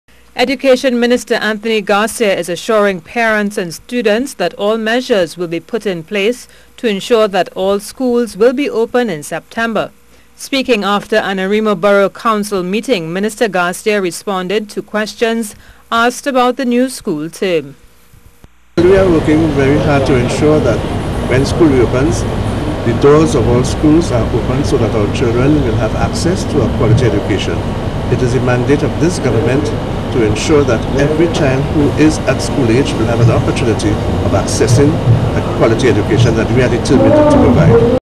Measures In Place To Ensure All Schools Open In September
Education Minister Anthony Garcia is assuring parents and students that all measures will be put in place to ensure that all schools will be open in September.

Speaking after an Arima Borough Council meeting on Monday, Minister Garcia responded to questions on preparations for the new school term.